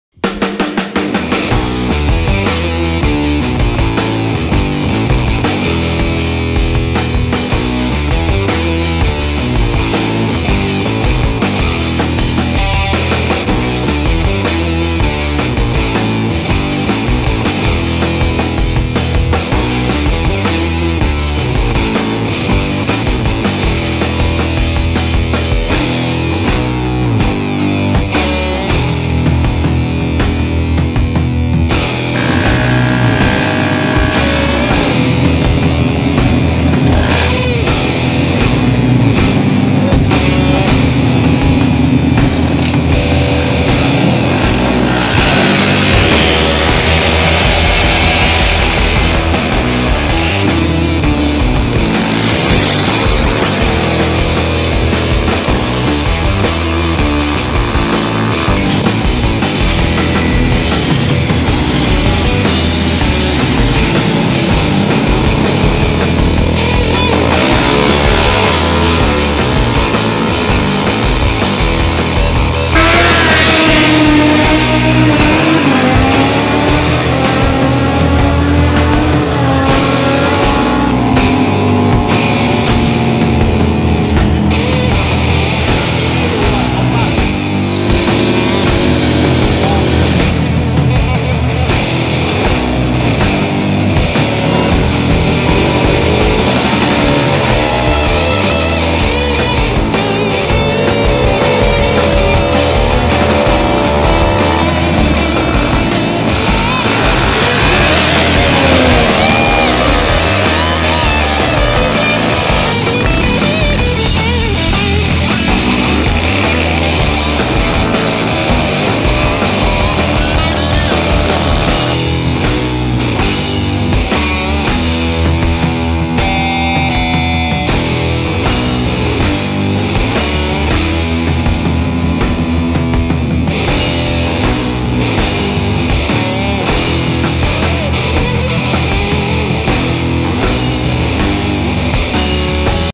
FASTEST HONDA PCX SCOOTER. DRAG RACE 402m BANGKOK DRAG AVENUE. FASTER THAN LAMBO & FERRARI! THAILAND
Drag race at Bangkok Drag Avenue.
Drag mobil Thailand
Honda PCX scooter Drag race at Bangkok - compillation
150cc only, but sometimes it takes less than 10s for 1/4 mile!
So, similar to Bugatti Veyron Super Sport  which costs about 2 mln $ ))